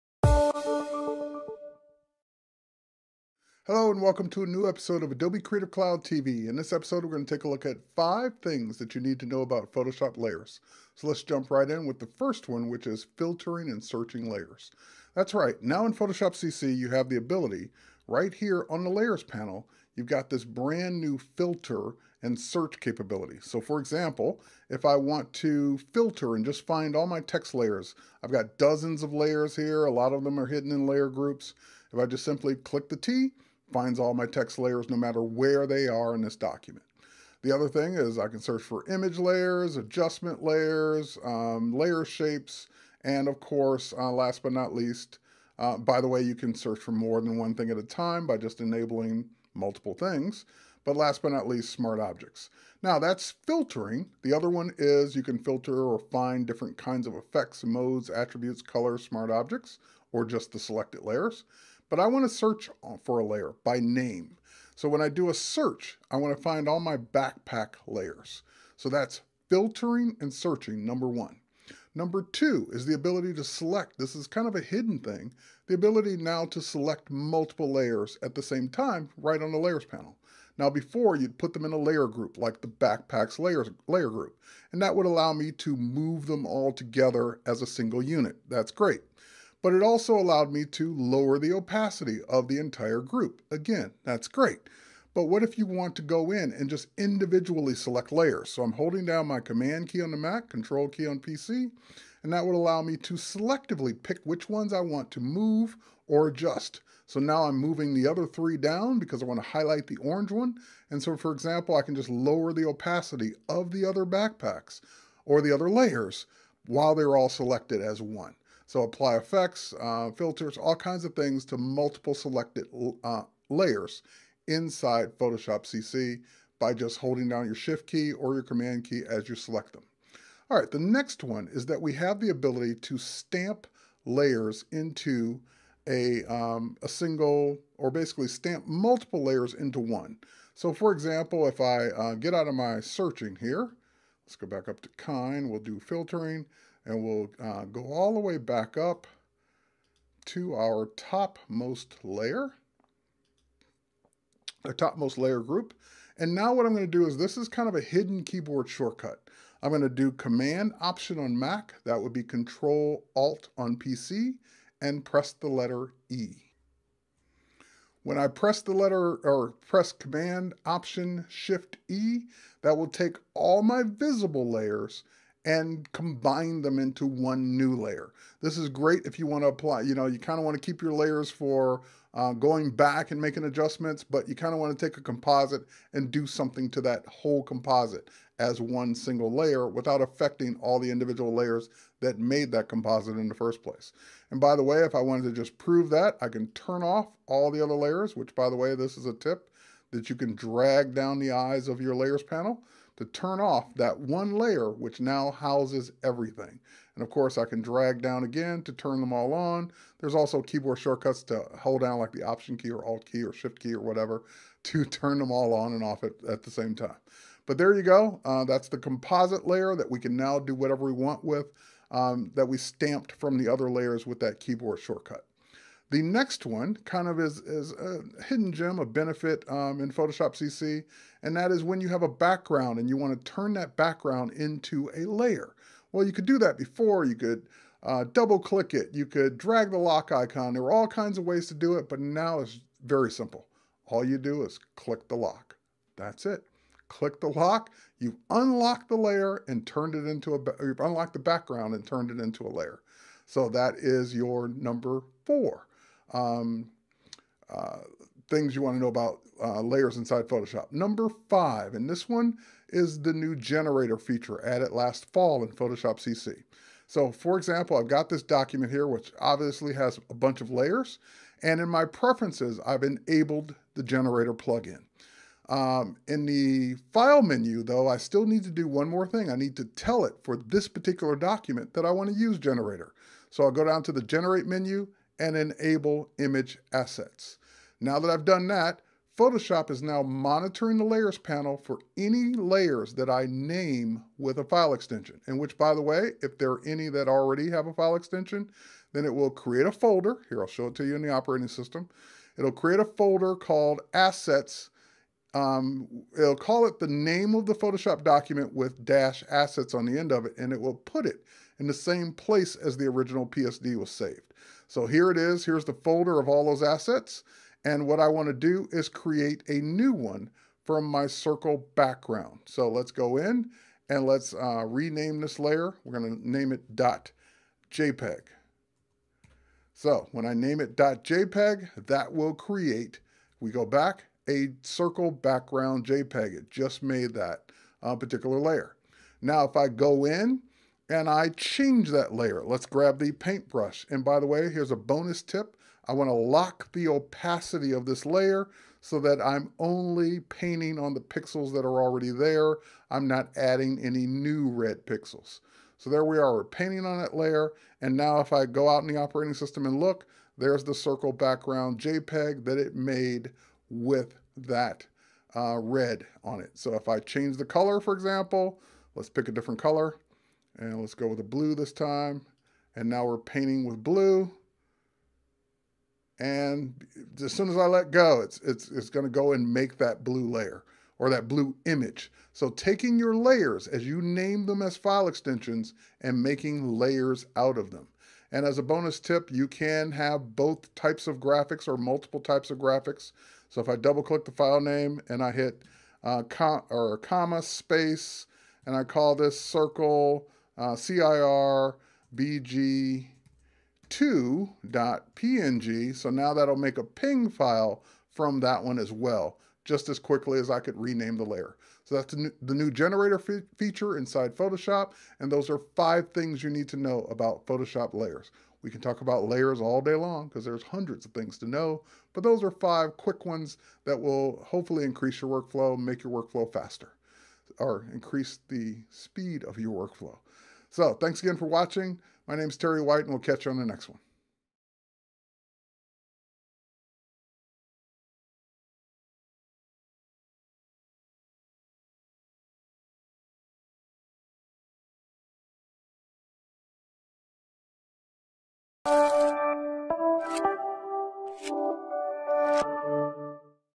5 điều bạn nên biết khi làm việc với Photoshop layers
Chia sẻ với các bạn về cách làm việc với các layers trên Photoshop (Ps). 5 cách mà chúng tôi muốn chia sẻ hôm nay hy vọng phần nào giúp được cho mọi người (đặc biệt là các newbie) có thể tối ưu hóa các thao tác của mình trên Ps một cách nhanh và hiệu quả nhất.

Video được thực hiện bởi Terry White